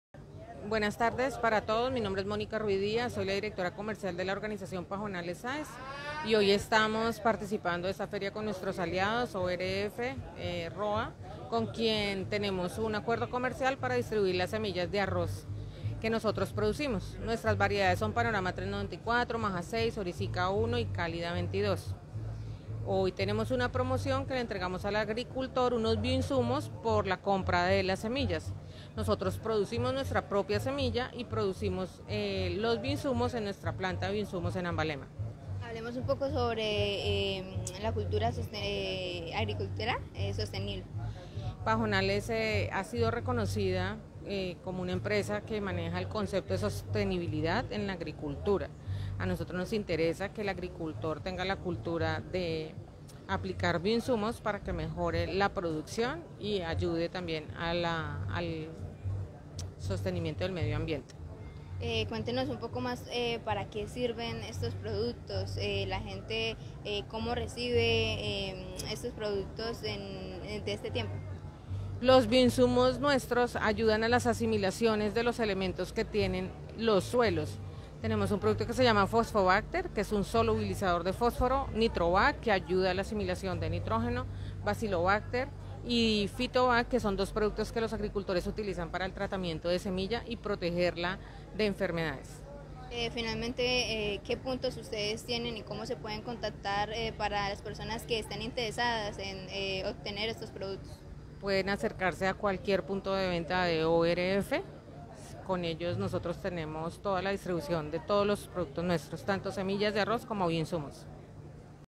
Monica Ruidiaz, directora comercial de la empresa Pajonales, desde el evento Agrolocura
Monica Ruidiaz, directora comercial de la empresa Pajonales, desde el evento Agrolocura que desarrola ORF S. A en Pore informó sobre la agricultura sostenible y los bioinsumos que producen.